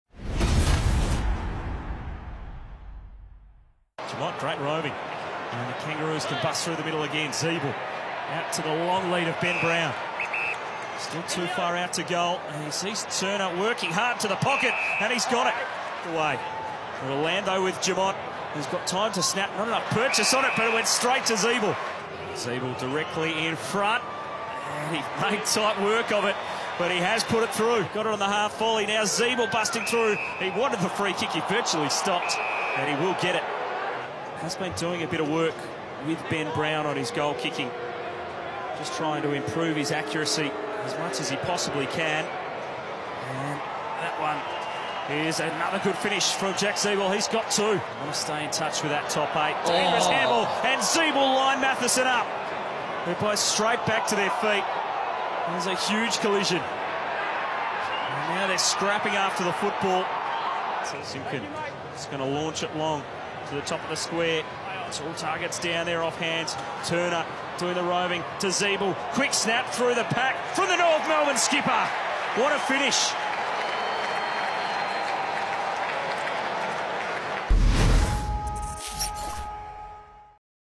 Jack Ziebell stands tall in Kangas' thriller (Round, 20, 2018)
Jack Ziebell kicked three goals and had 26 disposals in North's three-point win over the Lions.